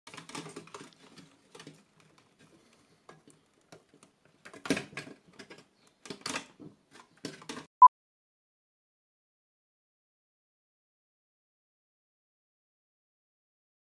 Disassemble 03 / Free Stock Footage (4K)
Description:
Close-up Footage of a man taking apart a motherboard. Motherboard repair. Main board repair. Disassemble a Computer. Remove a part. Removing a Cable. Assemble a Computer.